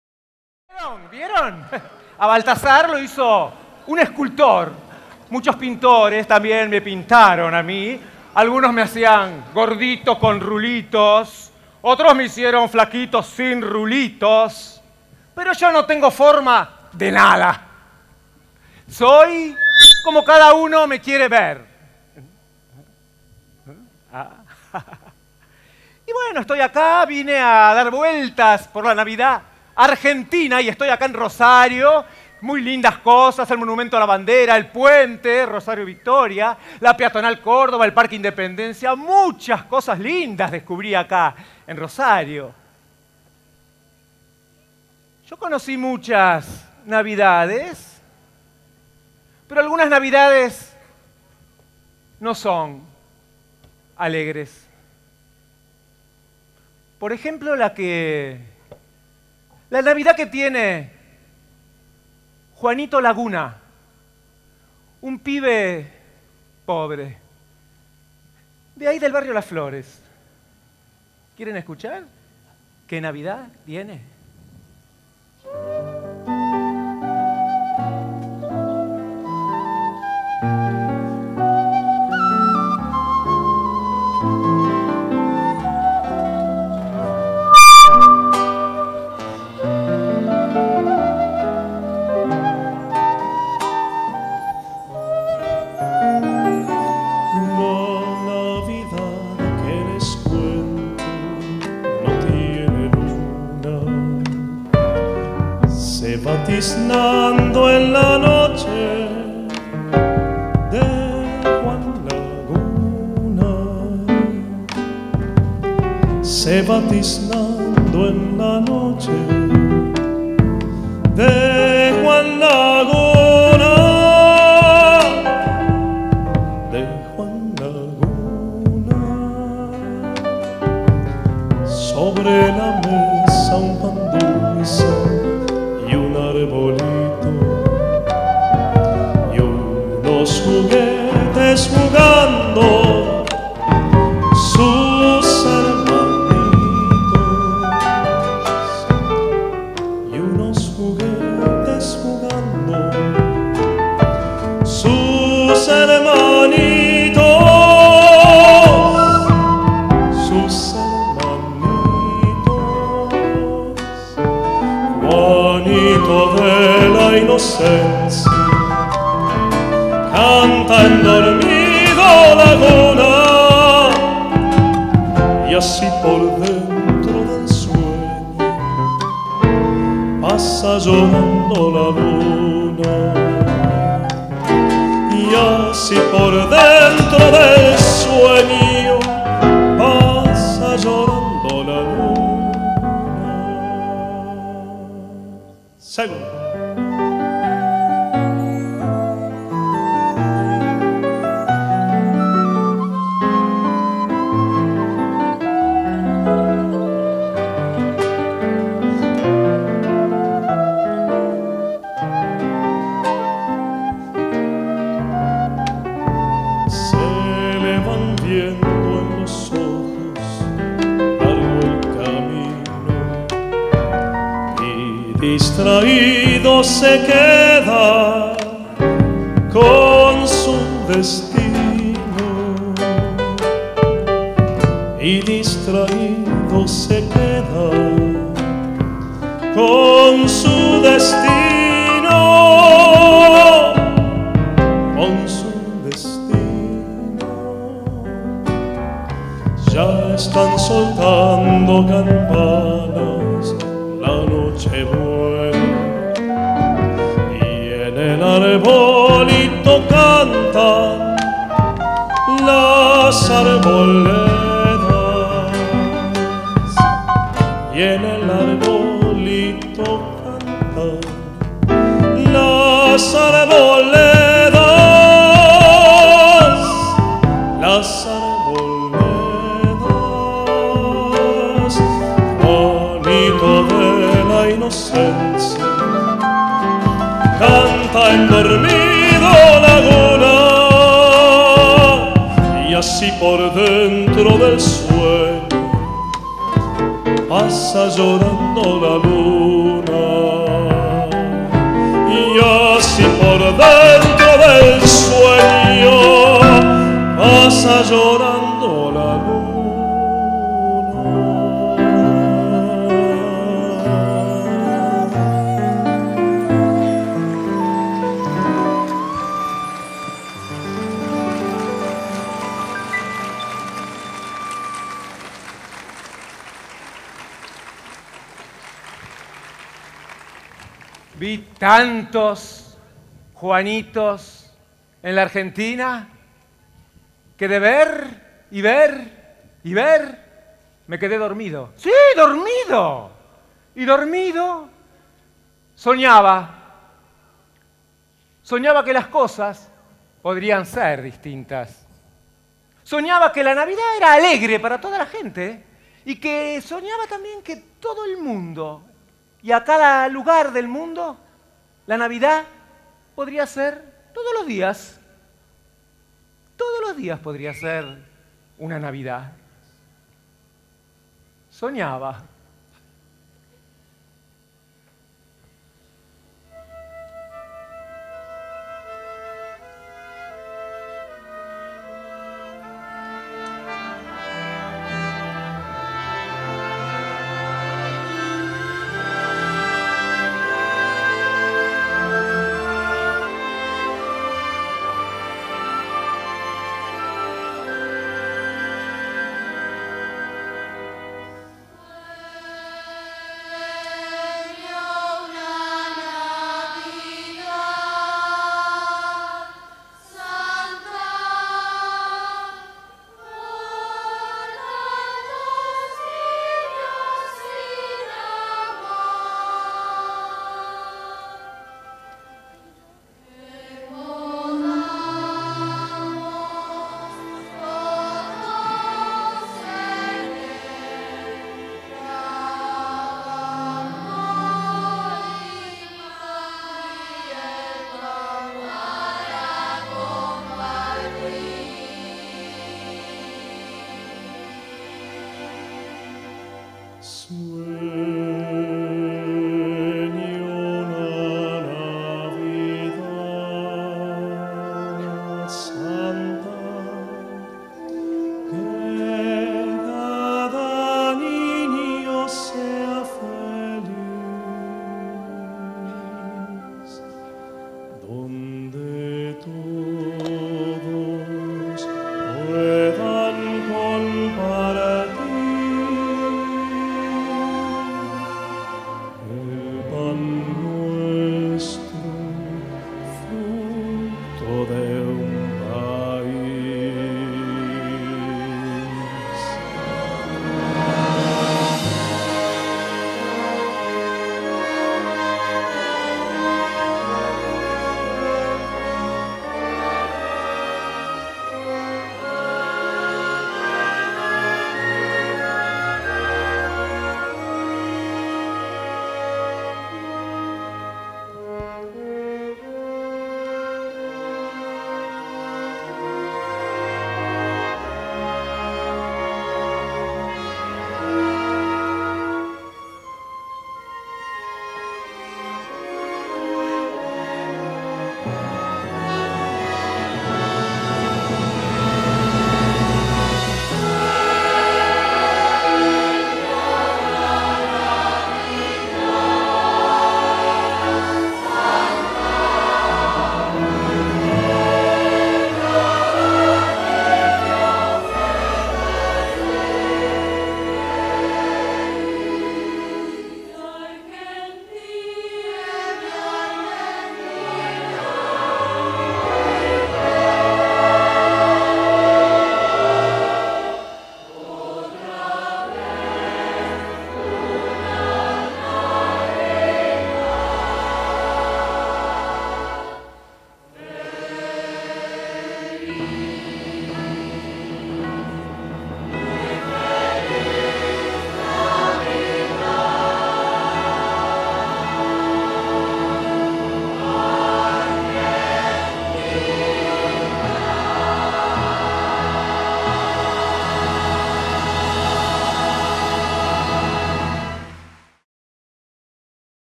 El día después de Navidad (6)
Espectáculo navideño en el Monumento a la Bandera, Rosario, Argentina. Idea, guión, producción artística y dirección: Juanjo Cura. Co-dirección: Maximilano Cañas. Ballet El Trébol. Angel Gabriel: Héctor Ansaldi (Piripincho). Coro de Niños de la Escuela Provincial de Música (Dir.: Mtro. Jorge Ferrari), Coro de Niños del Colegio San Ramón (Dir.: Carlos Maquiel), Coro de Niños y Adultos de Gdro. Baigorria (Dir.: Gerardo Belladore y Cintia Liberatti), Coro San Miguel (Dir.: José Salcedo). Solista infantil: Daiana Petit. Pesebre: Colonia de Verano del Colegio Maristas (Coord. Ma. Angélica Persiguini y Paula María Zapata). Producción ejectuiva: Banco Bisel.